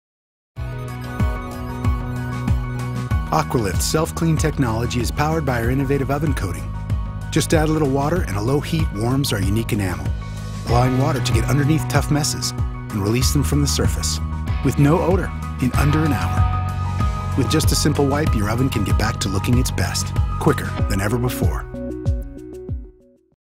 Port Huron Maytag Self-Cleaning Wall Oven
View the self-cleaning technology of a new Maytag AquaLift wall oven, available now at Miller Maytag Home Appliance Center.